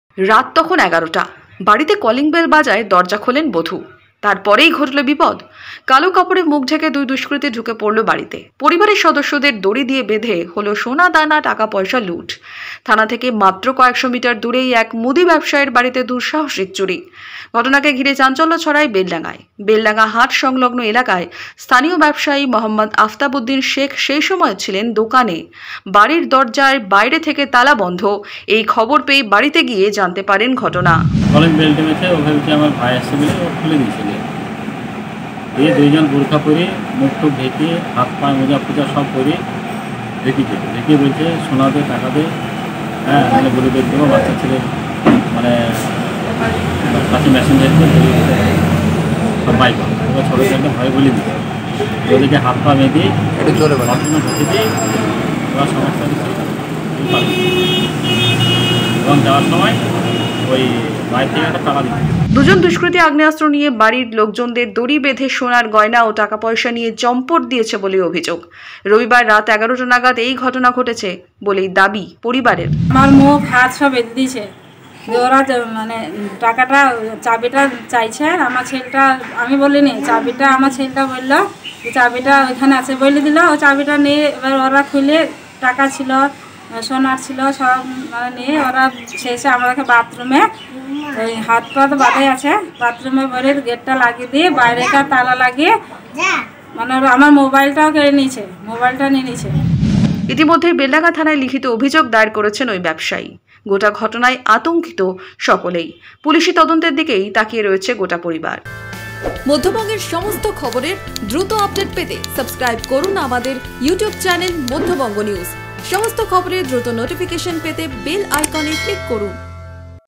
বেল বাজিয়ে ঘরে ঢুকল কালো কাপাড়ে মুখ ঢাকা দুষ্কৃতীরা, বেলডাঙ্গায় ভয়ঙ্কর ডাকাতি
Madhyabanga News